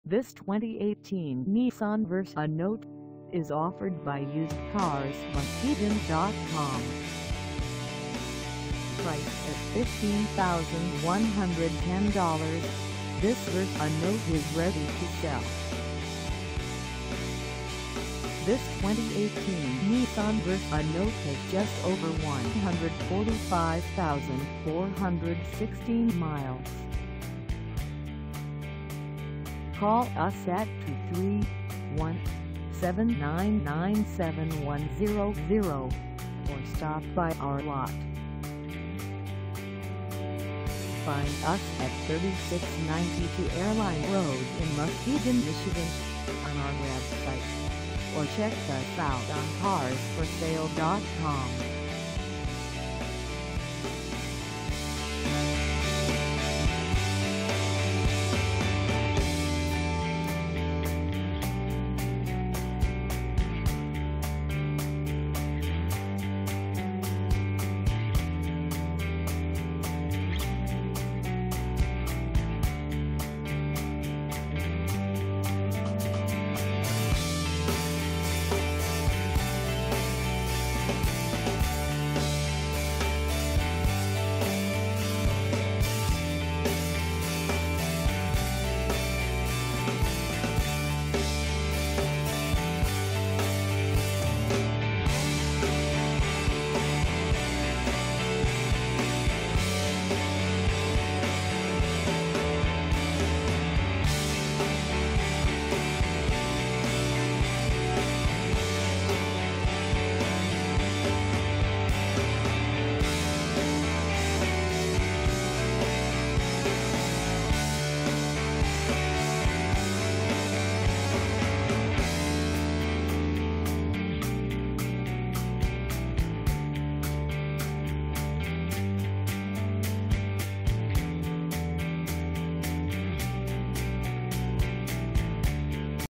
2018 Nissan Versa Note SV 4dr Hatchback for sale in Muskegon
This 2018 Nissan Versa Note SV 4dr Hatchback is for sale in Muskegon, MI 49444 at UsedcarsMuskegon.com.

Engine: 1.6L I4
Transmission: CVT
Color: Silver
Mileage: 145416
Contact UsedcarsMuskegon.com 3692 Airline Rd. Muskegon, MI 49444 to test drive this 2018 Nissan Versa Note SV 4dr Hatchback today.